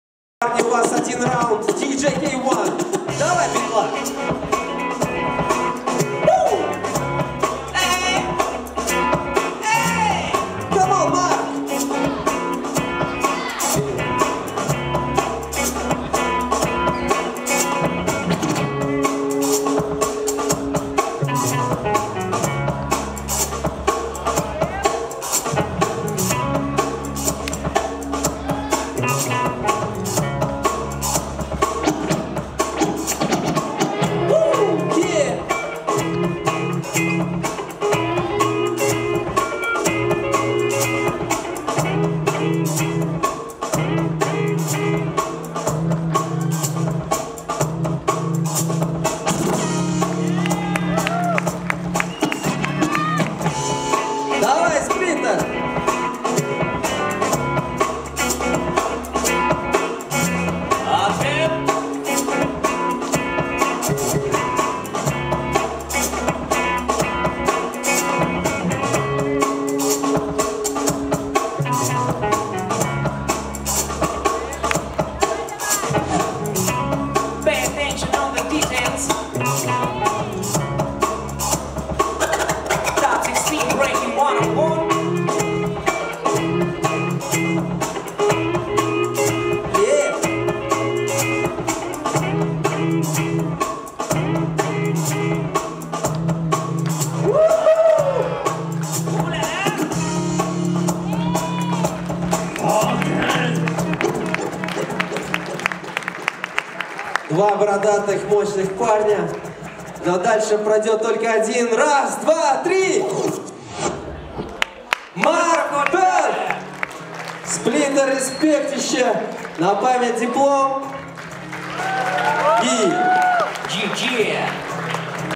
🙋‍♂️ BBOY BATTLE ↔ Splinter vs Mark Beard ↔ 1'8 ↔ COMBONATION X1 ↔
😜👉 Who is the winner? What do you think?

COMBONATION X1 at the festival met bboy Alik vs bboy Gryazniy and it was a hot battle! Nomination BREAKDANCE BATTLE is the nomination in which both dancers are not willing to give in! Each dancer has his own style of dance and battle! And only experience will show who truly deserves to be the first!